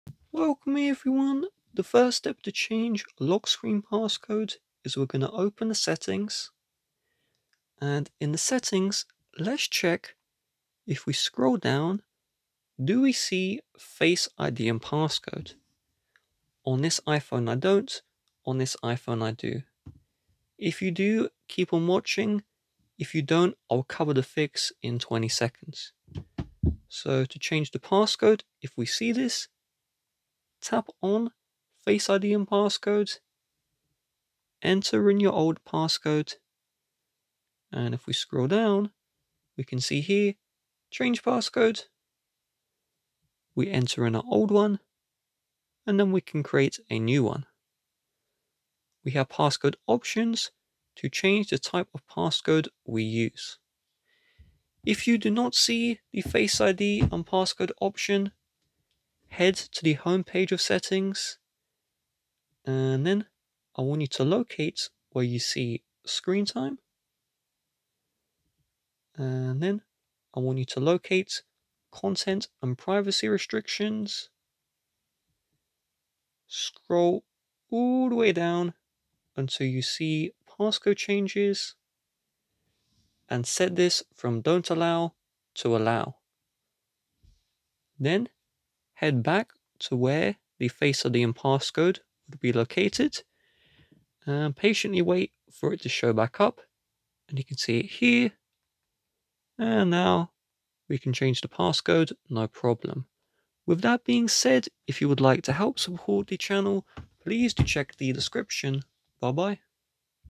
How To Change Lock Screen Password on iPhone 15 Pro Max
Learn how to change lock screen password on iPhone 15 Pro Max. You can have a 4-6 digit passcode, or an unlimited lengh letter and number password.